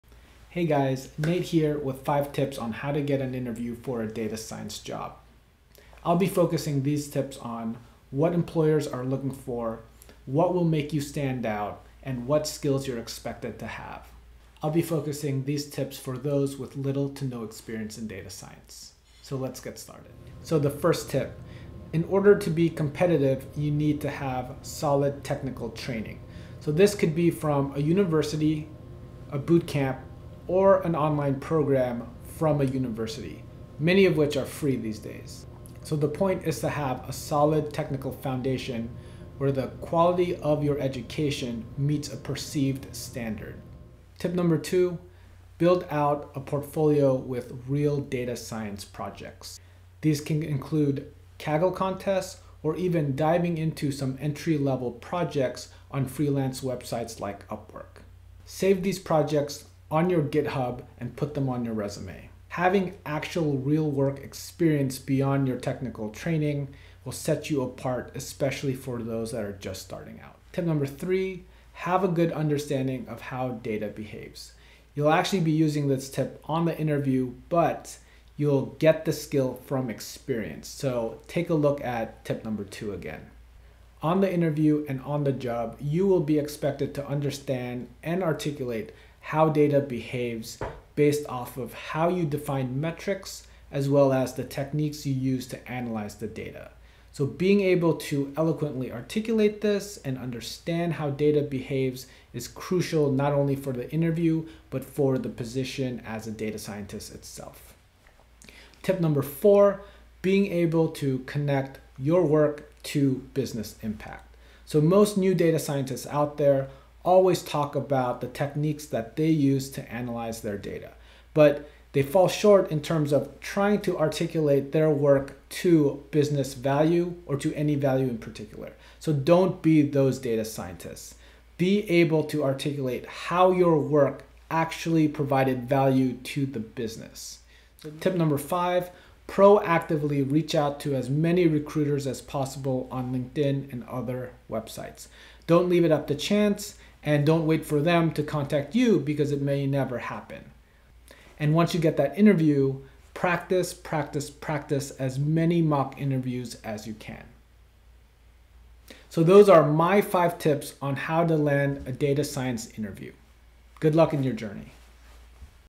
How To Get Your First Data Science Job Interview | Data Science Interviews Tip #2
This is data science interviews tip #2 and in this video, we'll talk on how to get your first data science job interview.

The extended explanations on how to get a data science interview without any professional experience from my conversation with the Python Programmer in this video